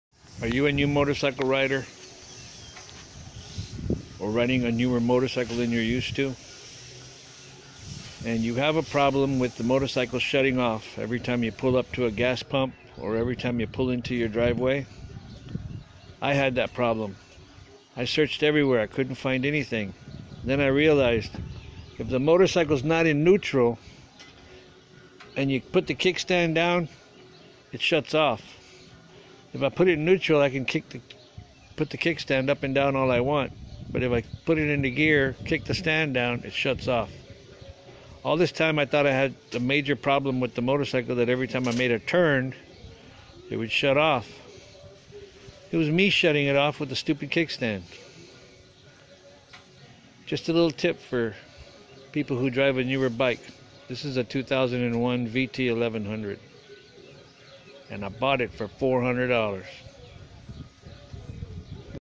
Motorcycle shuts of after a turn / stop. IT'S YOU NOT THE BIKE!
I had a problem that every time I made a turn and stopped at a gas pump or in my driveway my motorcycle would shut off and I could not figure out why. Watch this video to figure out why... it wasn't the motorcycle it was me